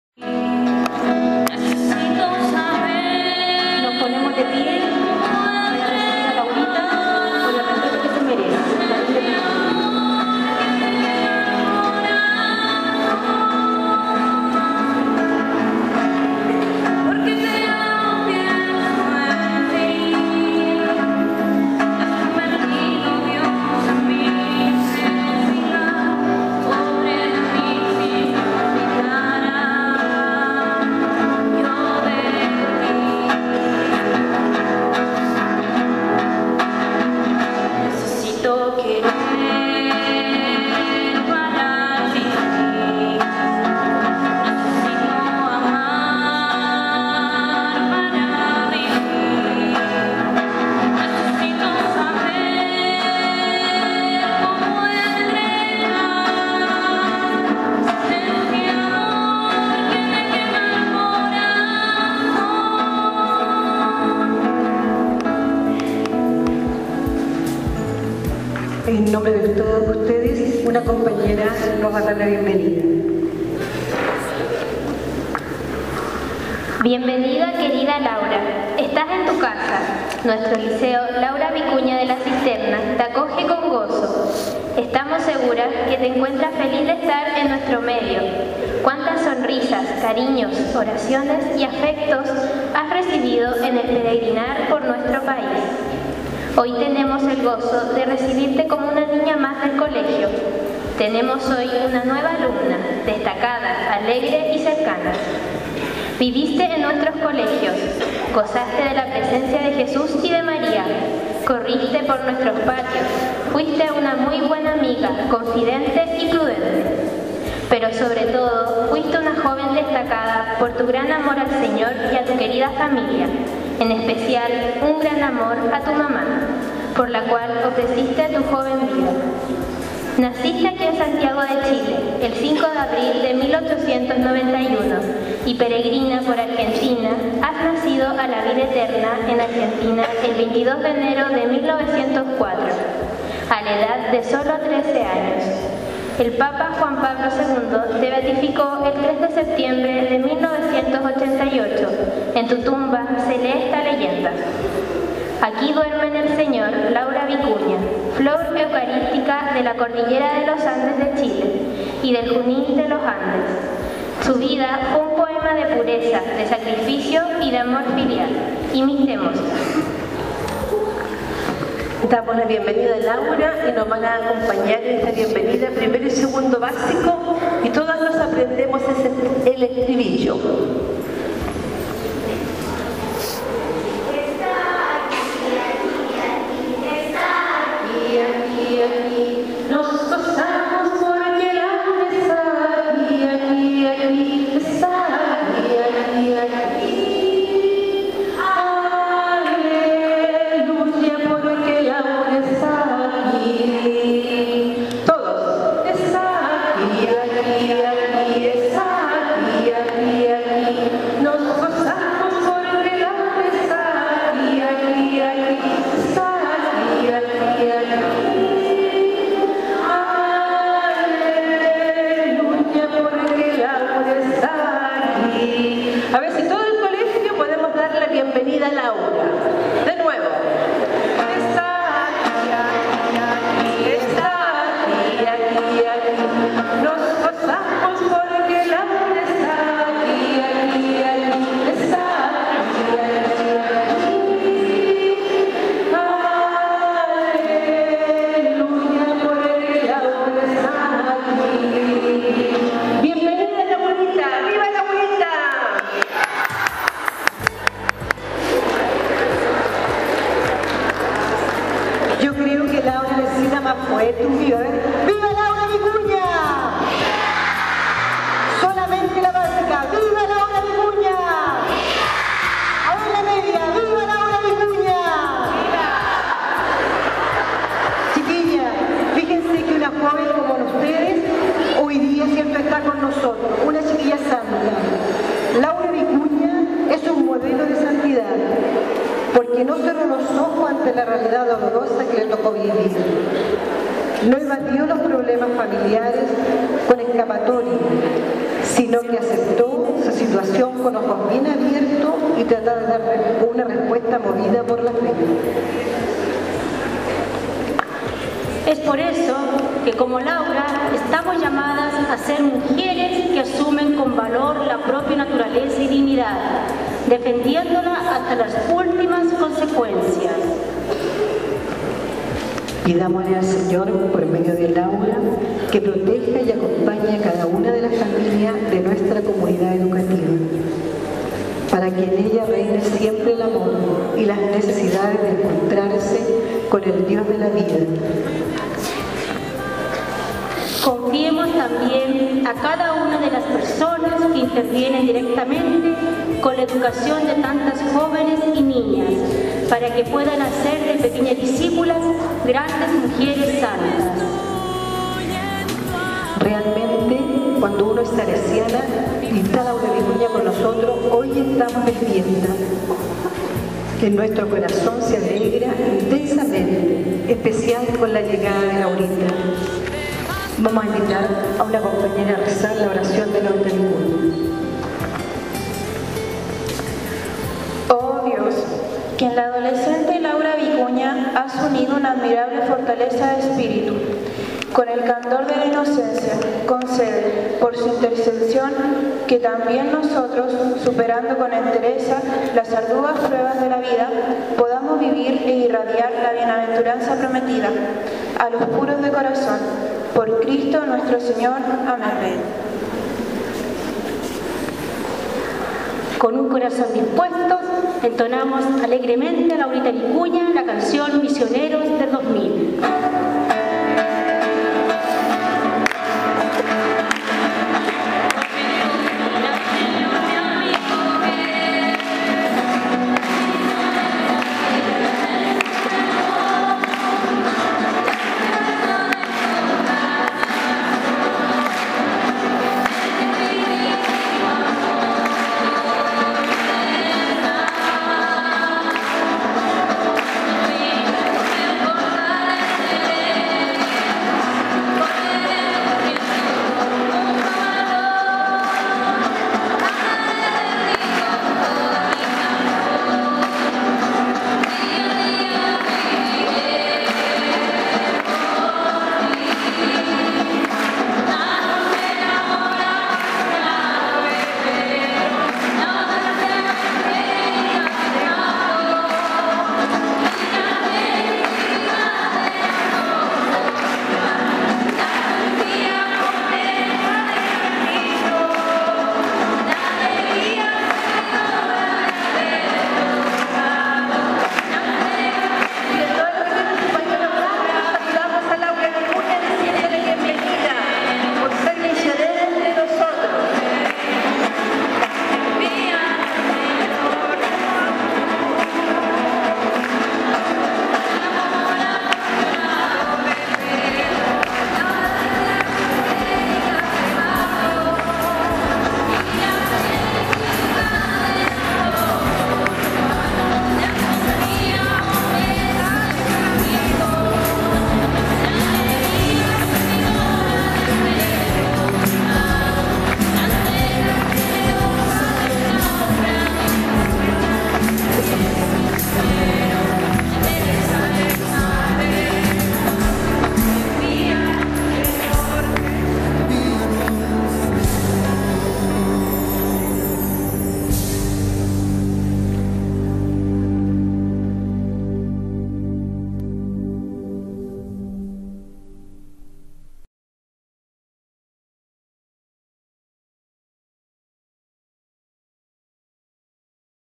Reliquia de Laura Vicuña es recibida por comunidad Liceo Laura Vicuña
Este lunes la comunidad LLV en pleno recibió la reliquia de Laura Vicuña, nuestra patrona ... disfruta el video!
Liceo Laura Vicuña
Puedes utilizar el video para los fines que estimes pertinentes solo recuerda nombrarnos
o en nuestro Facebook /liceolauravicunaoficial